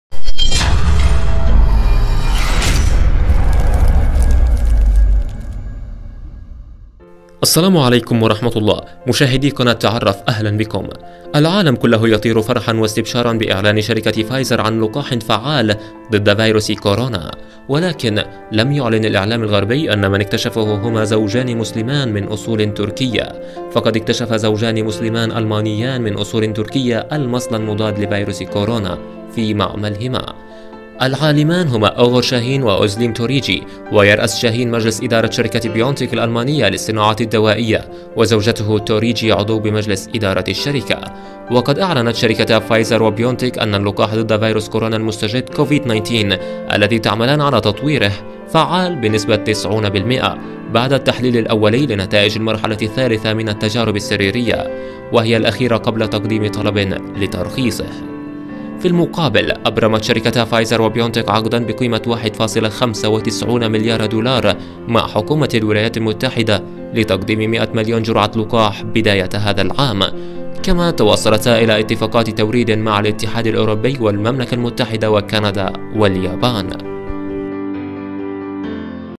زوجان مسلمان اكتشفا لقاح كورونا
تعرف على الزوجان المسلمان اللّذان اكتشفا لقاح كورونا والذي أعلنت عنه أمريكا..